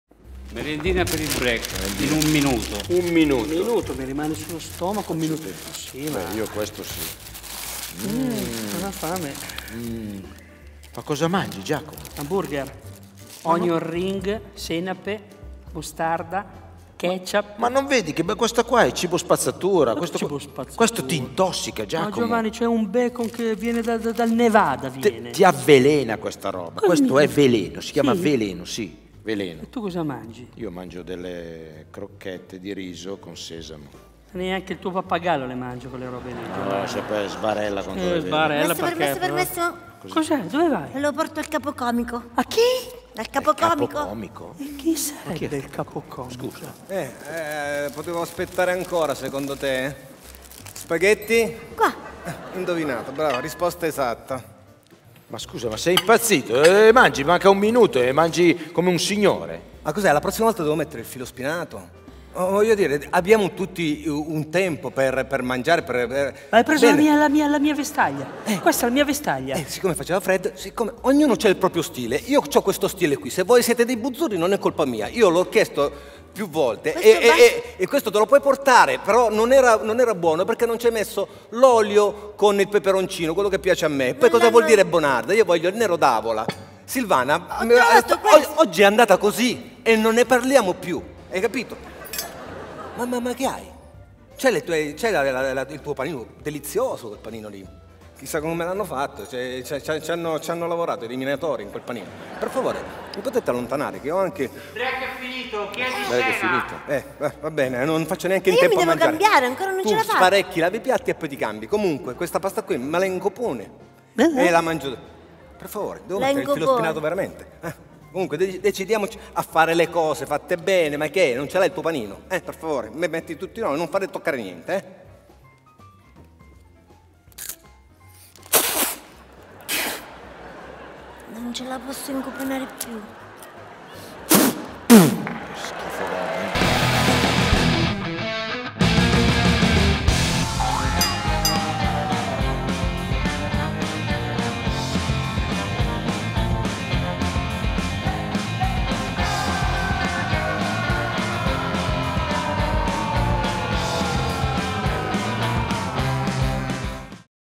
Il break #1 | Aldo Giovanni e Giacomo - Backstage di Ammutta Muddica
Aldo Giovanni e Giacomo cercano di godersi la breve pausa durante Ammutta Muddica! Guarda lo spettacolo QUI